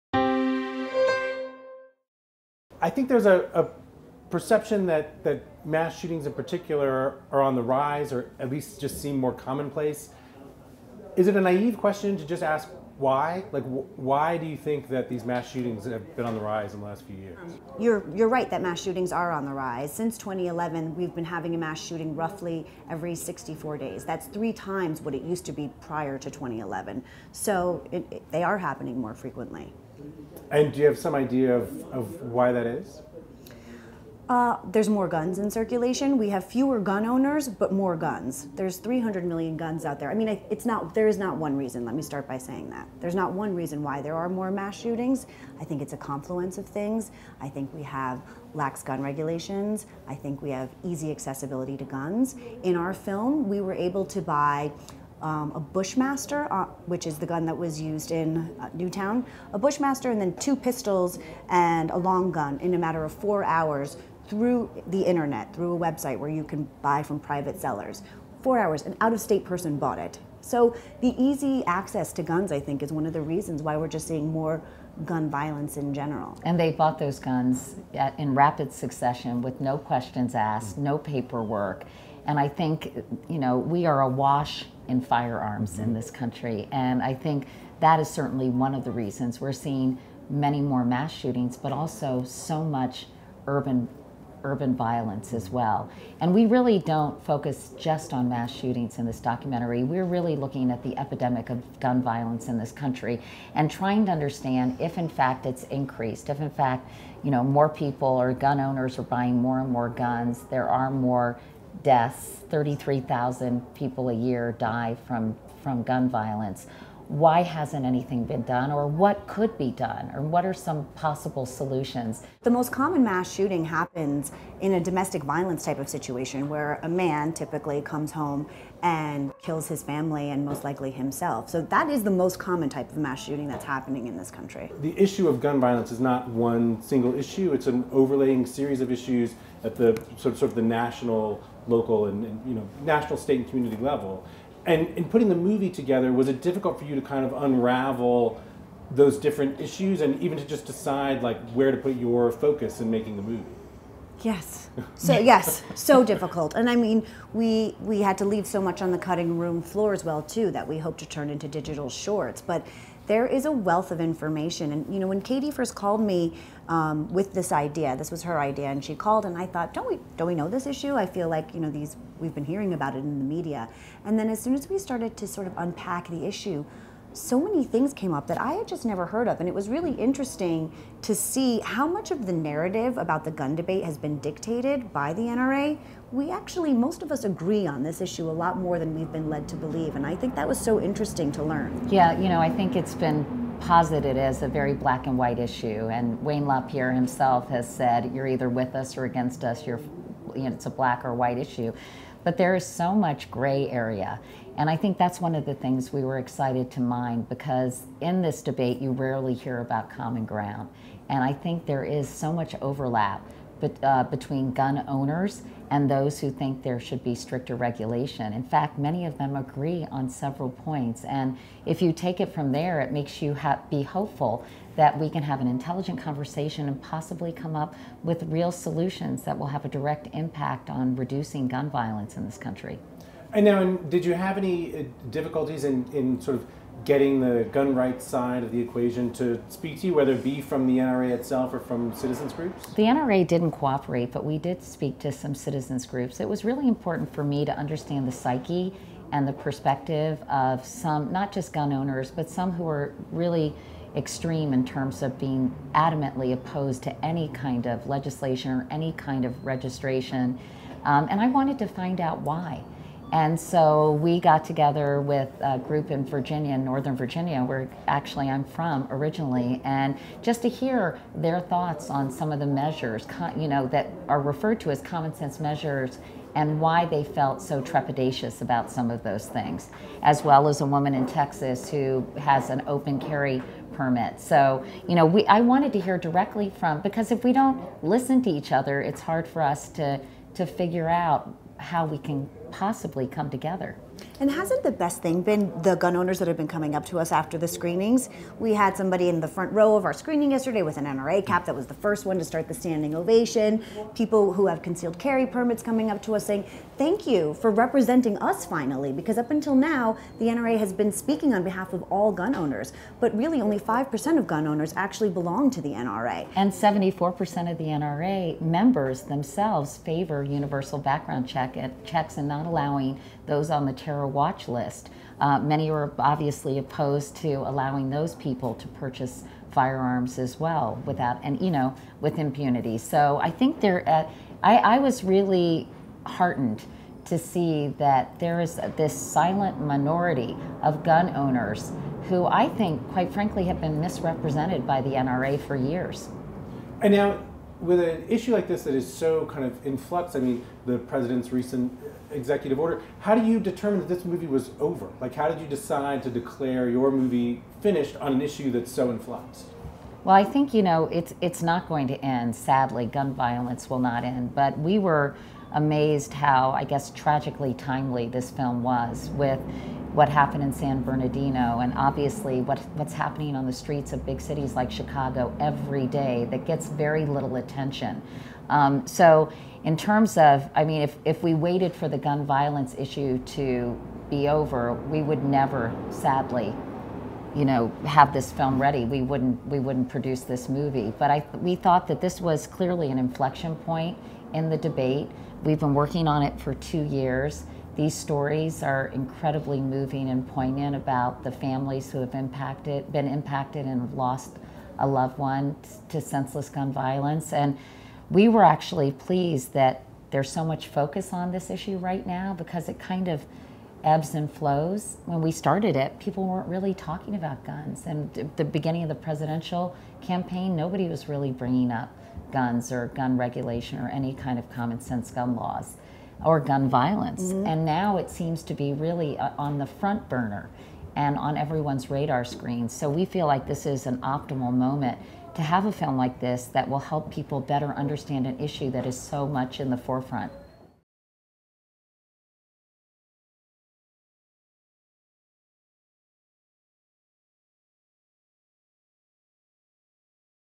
Sundance Film Festival 2016: Under the Gun
Filmmaker Stephanie Soechtig and executive producer Katie Couric look at guns and gun violence in the United States in the Sundance film, "Under the Gun."